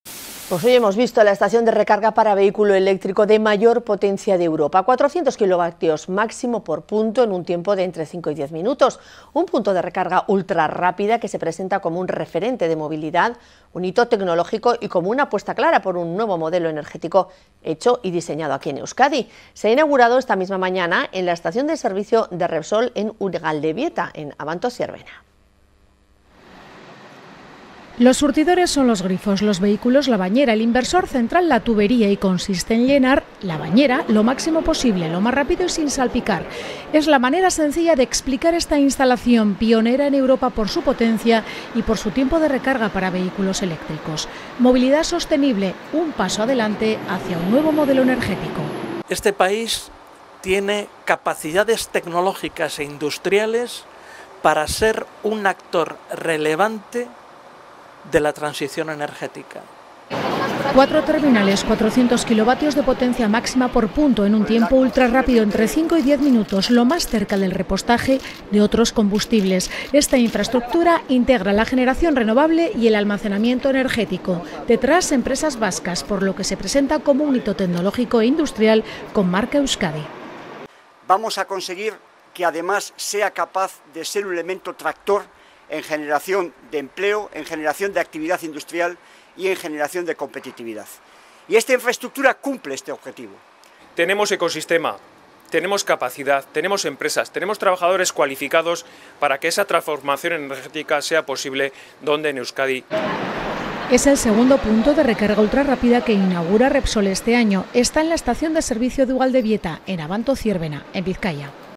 Repsol inaugura en Vizcaya la estación de recarga de mayor potencia de Europa (TVE1)
Repsol inaugura en Vizcaya la estación de recarga para vehículo eléctrico de mayor potencia de Europa (TVE1).

Repsolek Bizkaian inauguratu du ibilgailu elektrikoa kargatzeko Europako potentzia handiena duen hornitegia (TVE1).